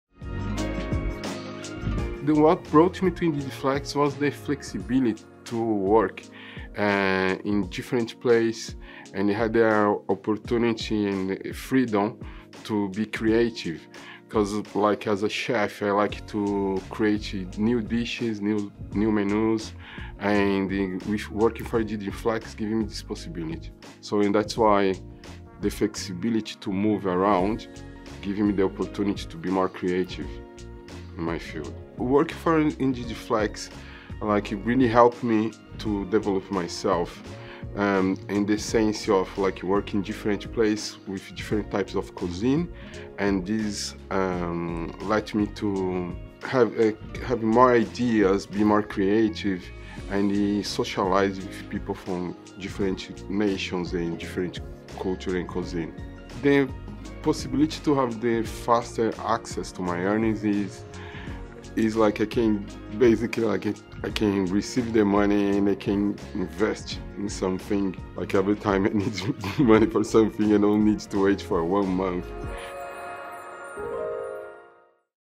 Indeed Flex Testimonial, Charlie
In Charlie's testimonial, he highlights how working with Indeed Flex has provided him the flexibility to unleash his creativity as a chef, allowing him to explore different cuisines, develop personally, and access his earnings instantly.